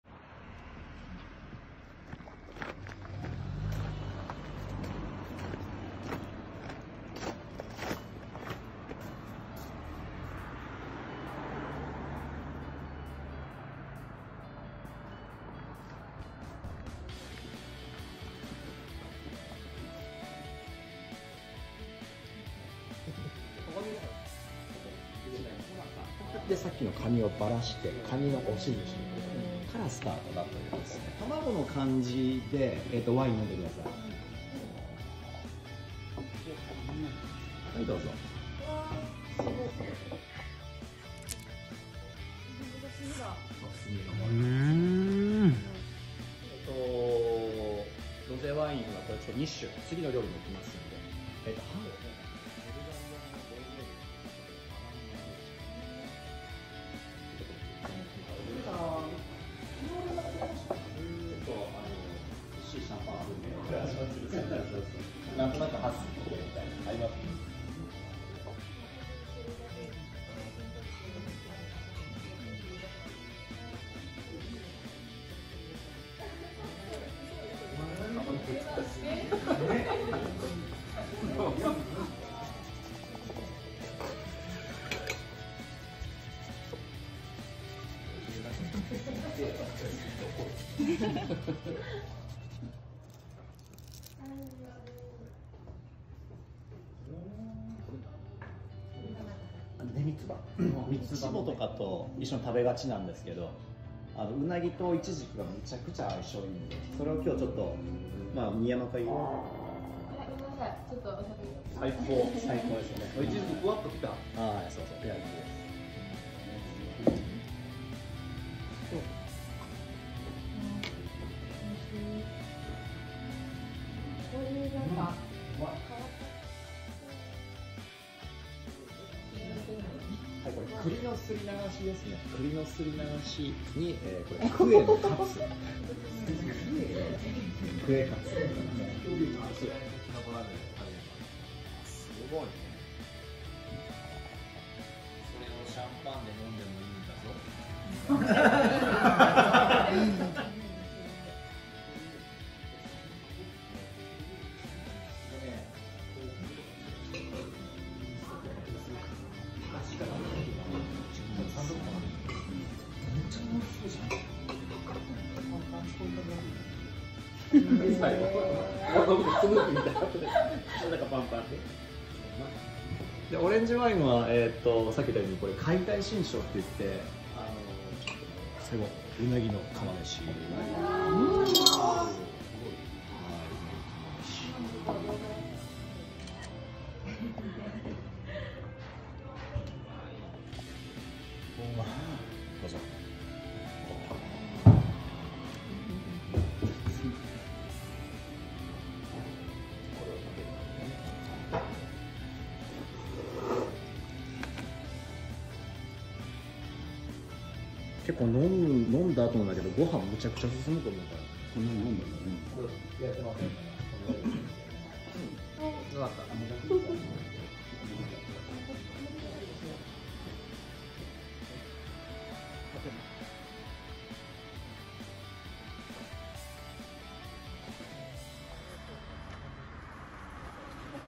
【名店】鰻英(旨すぎる鰻懐石)
予約困難‼️岐阜の名店【鰻英はなぶさ】鰻とワインのマリアージュ❗️この新世界はとても新鮮で楽しかった❗️一度は行くべき名店です。

予約可否
予約可
交通手段
岐阜駅よりタクシーで20分。

●鰻懐石御予約のお客様に限り
JR岐阜駅南口にて送迎サービスしております。

●GO TO EAT利用可能
営業時間
営業時間
日曜営業
定休日
《火曜日》